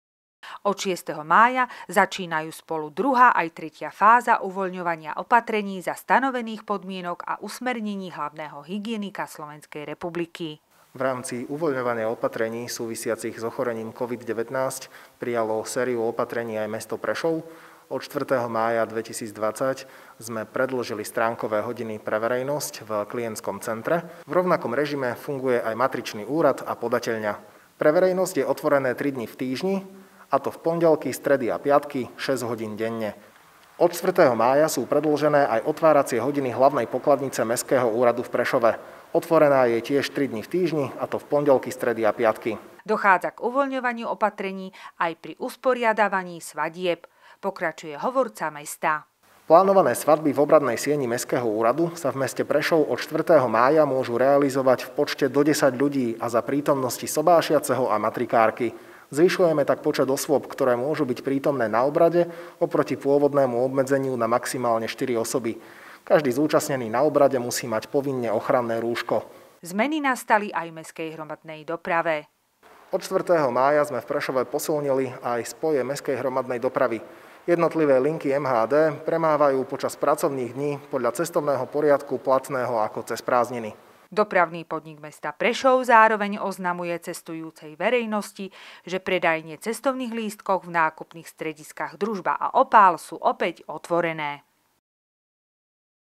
Uvoľňovanie opatrení
Od 6. mája začínajú spolu druhá aj tretia fáza uvoľňovania opatrení za stanovených podmienok a usmernení hlavného hygienika SR. Reaguje na to aj mesto Prešov.